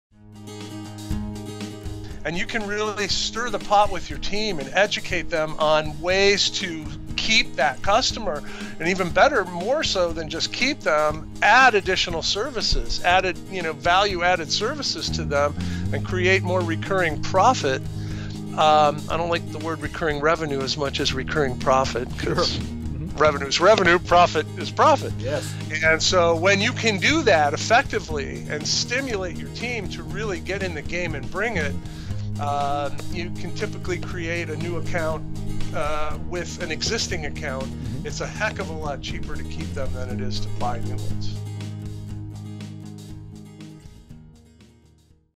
Bob Harris - Stir the pot (ItsPayd Live 01 - Clip1)
Bob Harris talks with Ken Green and Matt Unwin about Attrition, mistakes to avoid, and tip for retention.